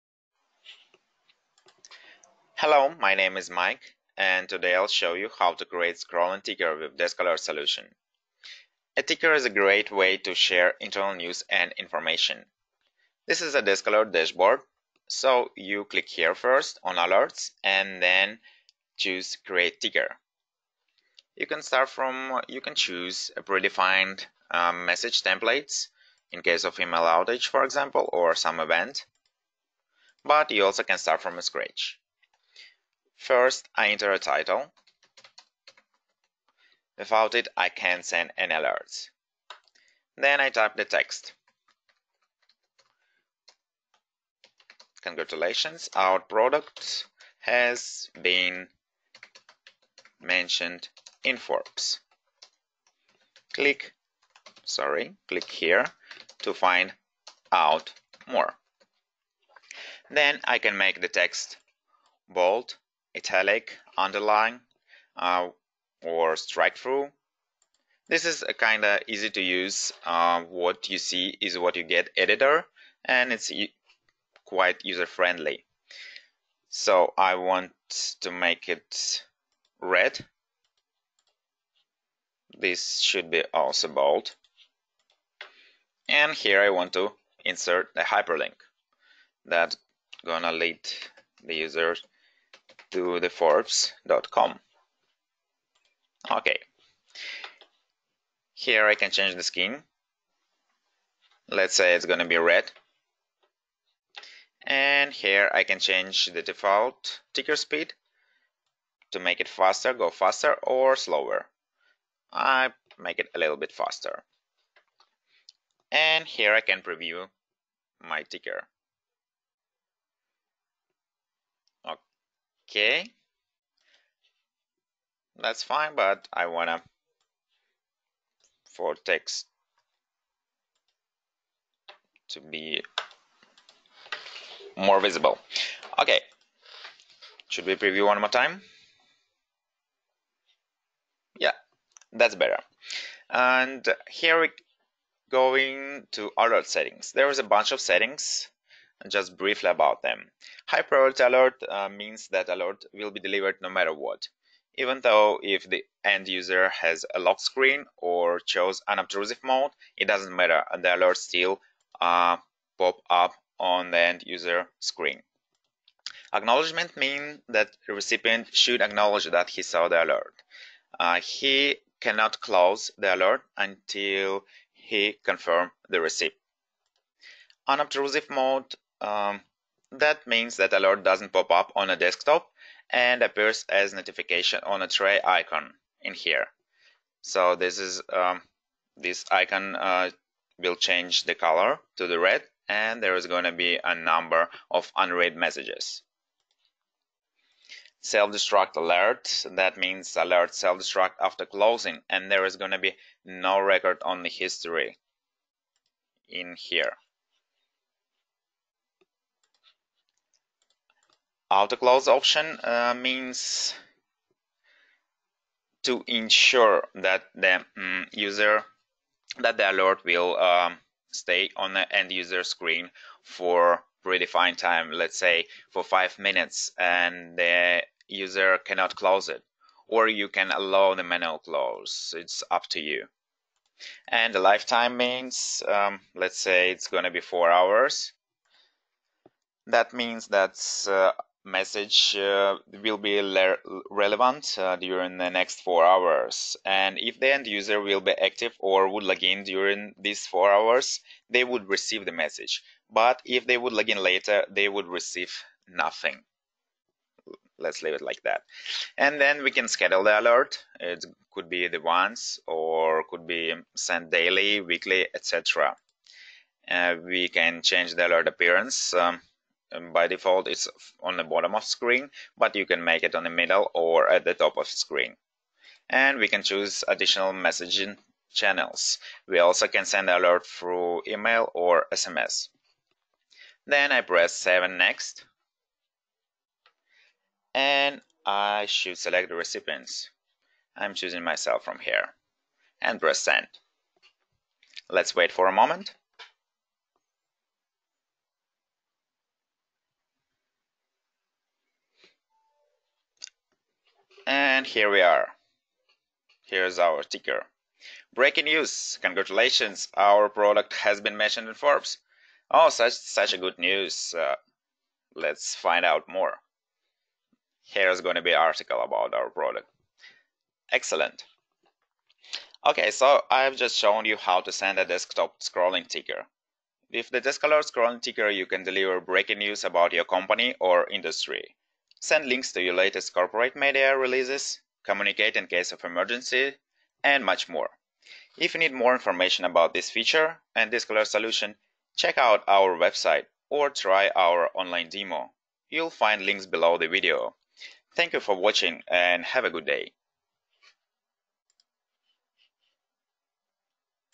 DeskAlerts Desktop Ticker Software
Use popup ticker to notify all employees -- your messages won't be missed or ignored. This video will show you how to use DeskAlerts desktop ticker.